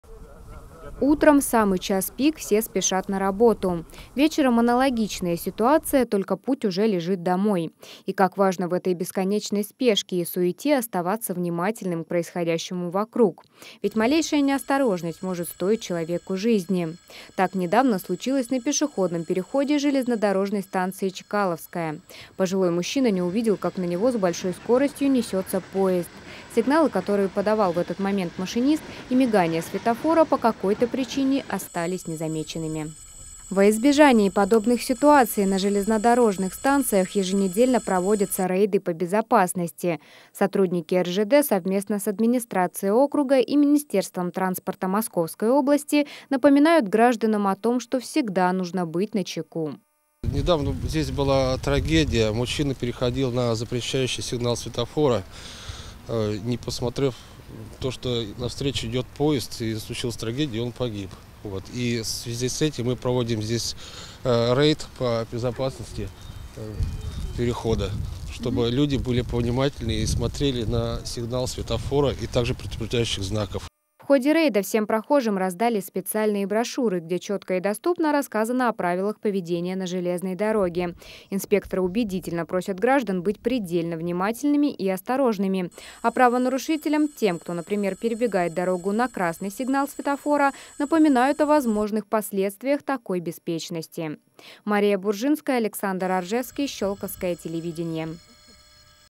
О безопасности на железной дороге
Рейд по безопасности на железных дорогах прошел на станции Чкаловская. Пешеходам, спешащим на электричку, и переходящим железнодорожные пути, раздали тематические листовки с правилами, соблюдая которые, человек останется в целости и невредимости.